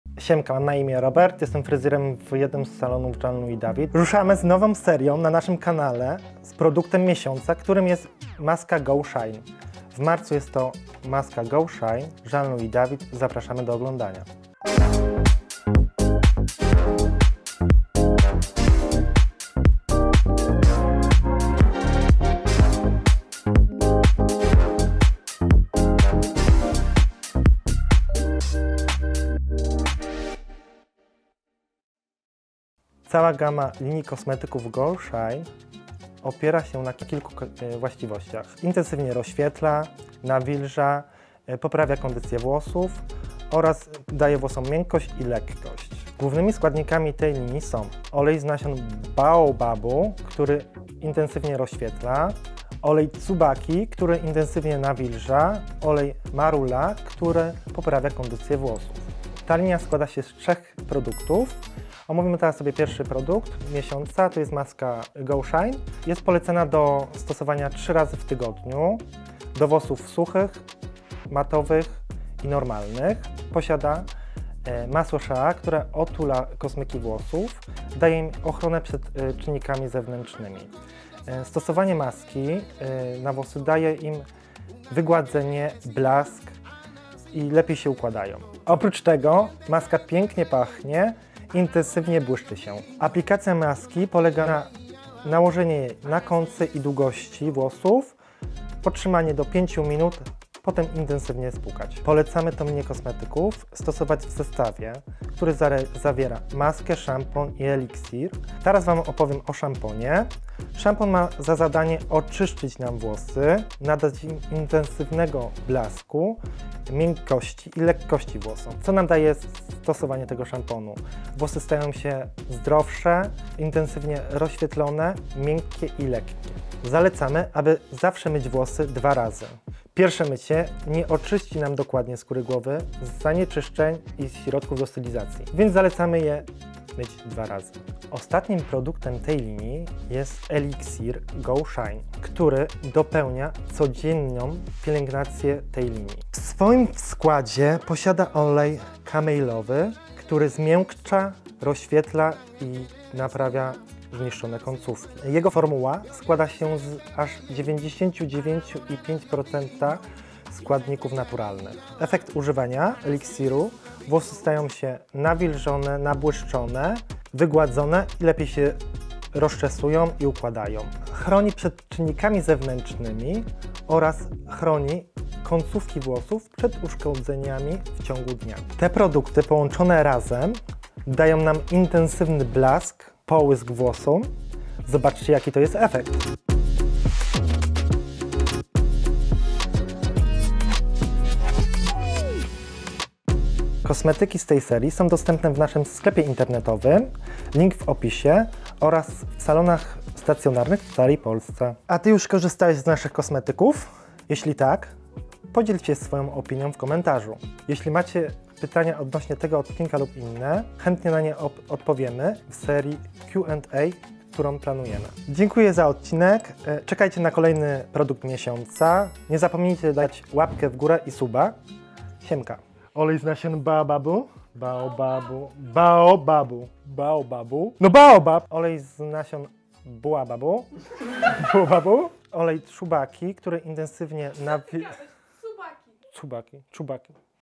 PRODUKT MIESIĄCA - maska JLD Go Shine!
Oto pierwszy odcinek z naszej najnowszej serii „Produkt miesiąca”! W marcu jest to maska JLD Go Shine. Jeżeli chcesz poznać jej skład, właściwości, działanie - obejrzyj ten film! Jest to idealny produkt dla Ciebie, jeżeli pragniesz maksymalnego rozświetlenia. Najlepiej stosować całą serię w pakiecie - szampon, maska i eliksir

1. Maska JLD Go Shine
2. Szampon JLD Go Shine
3. Eliksir JLD Go Shine

Czy warto stosować profesjonalne kosmetyki zamiast drogeryjnych preparatów? Jak najbardziej! Dlaczego? Głównie ze względu na skład, zdrowszy i skutecznie działający na naszych kosmykach. W linii produktów Jean Louis David Go Shine dostępnej w sklepie internetowym i salonach stacjonarnych w całej Polsce dominują oleje: marula, tsubaki i z nasion baobabu. Zapewniają głęboki połysk i odżywienie. Dodatkowo produkty nie zawierają parabenów, siarczanów i silikonów.

00:00 Wstęp
00:33 Właściwości linii JLD Go Shine
00:47 Główne składniki linii JLD Go Shine
01:06 Maska JLD Go Shine
01:50 Szampon JLD Go Shine
02:30 Eliksir JLD Go Shine
03:00 Efekt działania kosmetyków